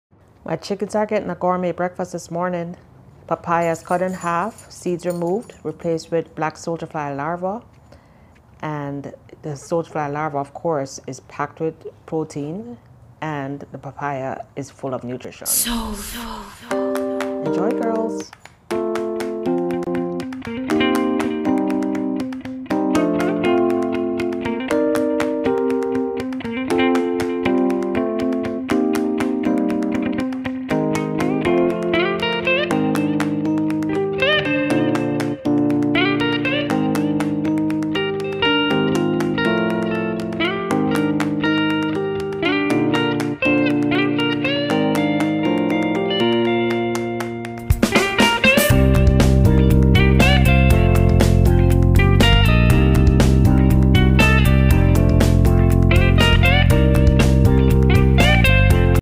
Gourmet breakfast chicken treat - Papaya stuffed with black soldier fly larvae
I try to supplement my chickens feed with some yummy and nutritious treats that also support their health.

today they are getting papaya with black soldier fly larvae